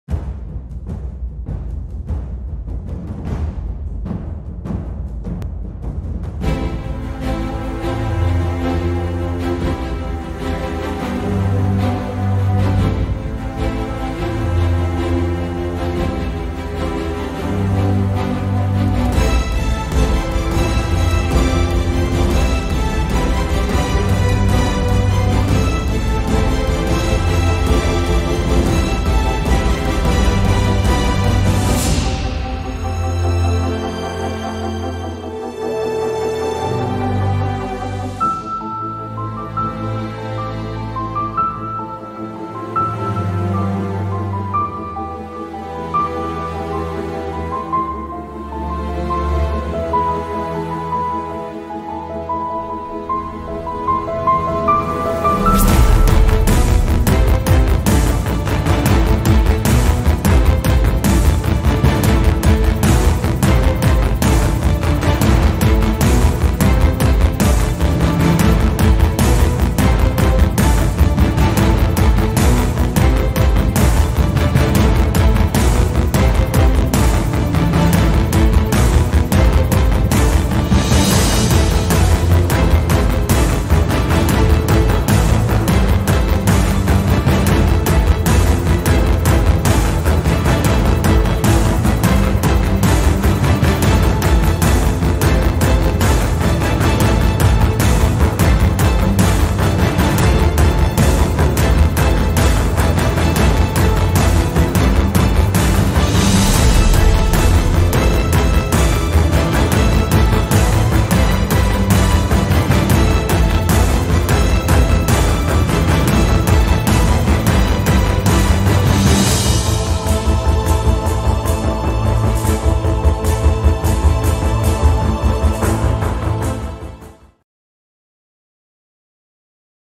Season I, US Cyber Team: European Cyber Security Challenge Austria, Vienna Starting Lineup
Cheer on the Season I US Cyber Team as they head to Vienna, Austria, to participate in the European Cyber Security Challenge (ECSC) from September 13 to 16, 2022.